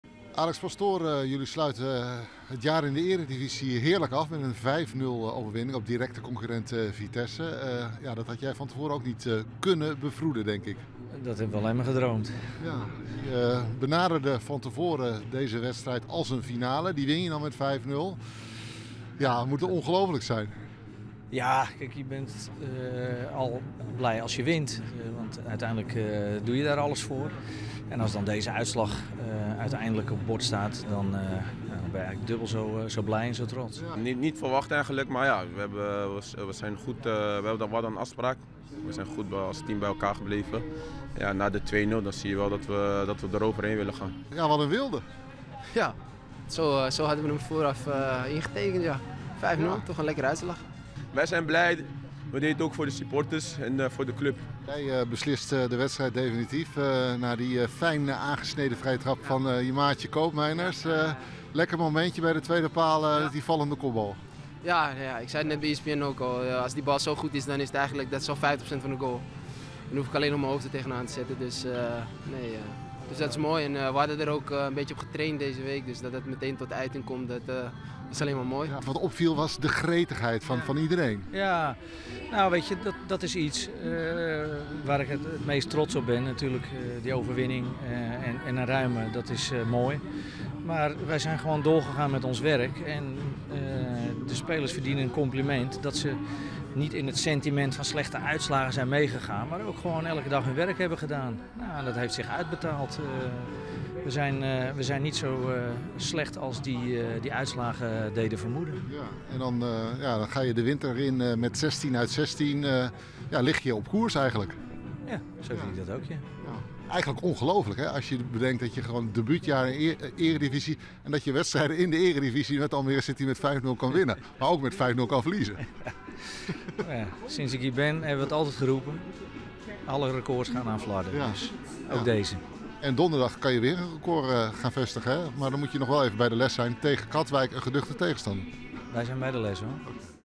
Almere City wint met 5-0 van Vitesse: reactie trainer en spelers | Omroep Flevoland
Almere City FC heeft zondagmiddag met 5-0 gewonnen van Vitesse. Het was de laatste competitiewedstrijd van dit jaar. City trok de regie in de eerste helft steeds meer naar zich toe,  na rust kwam Vitesse niet terug.